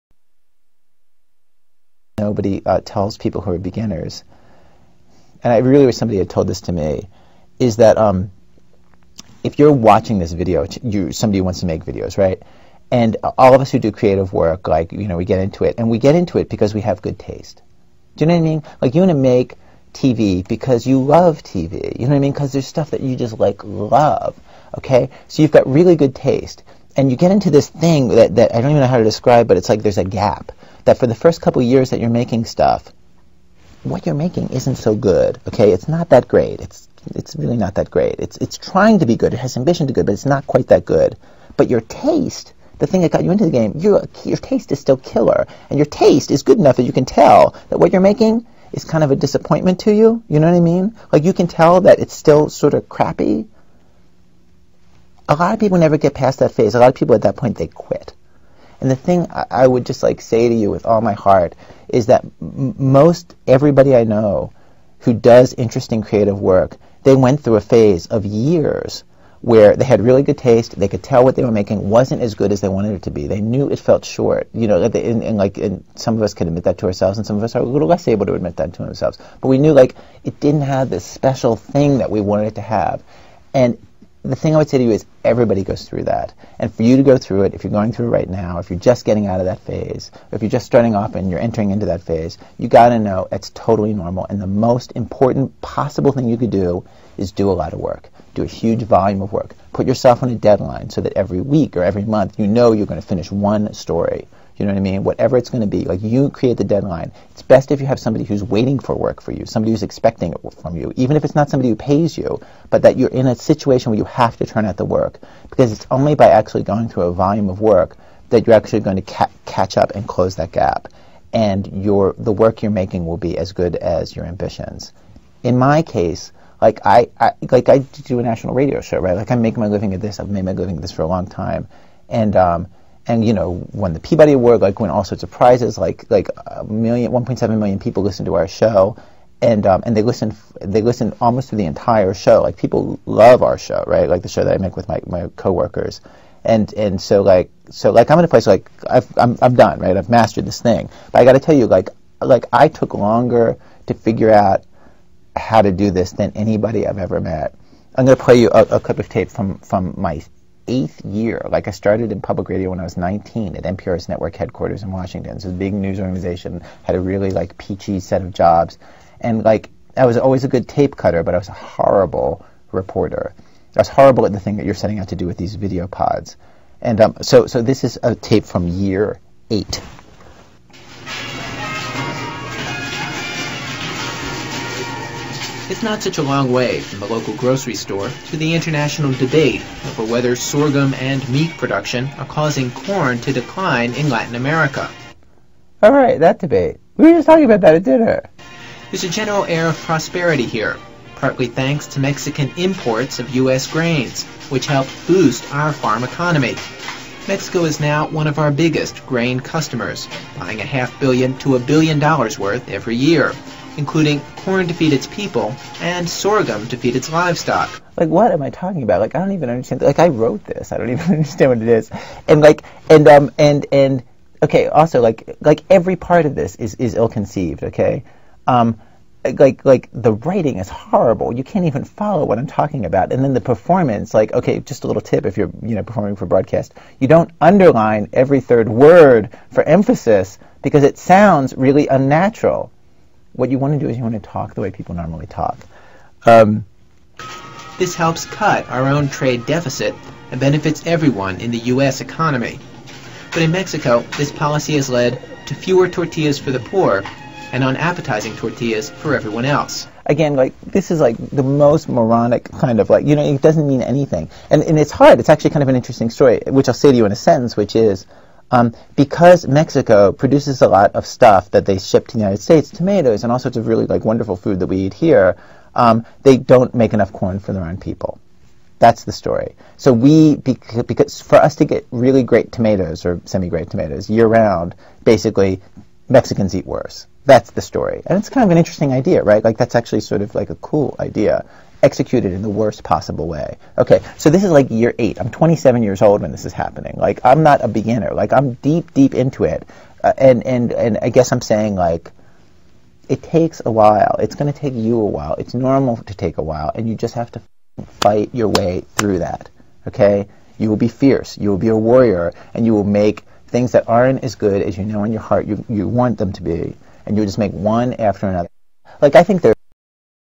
Ira Glass on Storytelling 3
Ira Glass from This American Life talking about story telling. Part 3, On good taste and falling short.  This is a wonderful episode on how we all go through phases where we know our work is not up to par, even though we are trying so hard to make it perfect.  Keep doing your work, keep focused and dont give up!